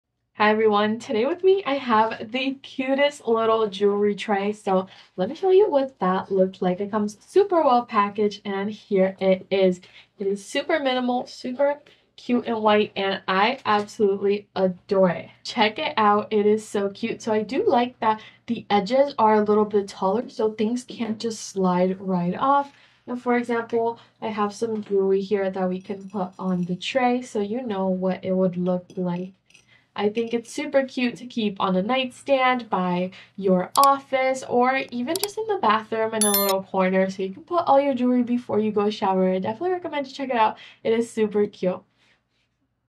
Jewerly Tray Review
Discover the beauty and functionality of the conviv Ceramic Jewelry Tray, a versatile piece of decor that adds a touch of elegance to your space. Whether you use it as a jewelry organizer, a perfume trinket dish, or a stylish catch-all for keys and rings, this charming ceramic tray is not only practical but also a delightful room accent. It's the perfect gift for bridesmaids, birthdays, Mother's Day, or as a warm housewarming, Thanksgiving, or Christmas present for the women in your life.
💍 Key Features:
* Chic Aesthetic: The Ceramic Jewelry Tray features a minimalist and chic design that complements any room decor, from bedrooms to bathrooms, vanities to living spaces.
* Versatile Use: Use it as a jewelry holder, a trinket dish, a key organizer, or a vanity decor piece – its versatility knows no bounds.
* Thoughtful Gift: Show your appreciation to bridesmaids, friends, family, or loved ones with a gift that combines aesthetics and functionality.
* High-Quality Material: Crafted from durable ceramic, this tray is built to last and adds a touch of sophistication to your space.
* Warm House Decor: The tray's warm and inviting design makes it an ideal addition to your home, providing both function and charm.
* Compact and Practical: Its compact size ensures it fits seamlessly on any surface, from dressers to nightstands, making it easy to keep your essentials organized.
Elevate your room decor and add a touch of elegance with the conviv Ceramic Jewelry Tray. It's more than just a tray; it's a beautiful and thoughtful gift that makes every day a little brighter.